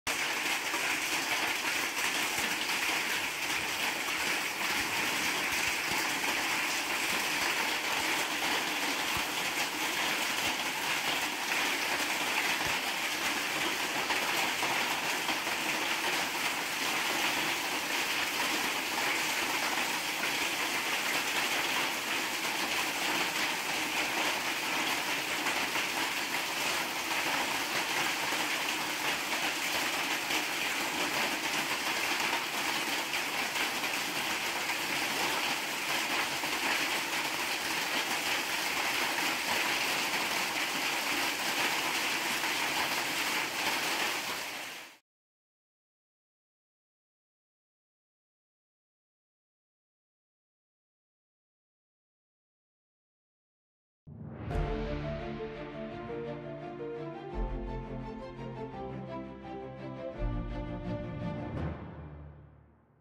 В Калужской области ударил град размером с орех
Крупный град ударил 8 июня в Сухиничах. Сообщают, что в некоторых местах он даже скопился наподобие снега.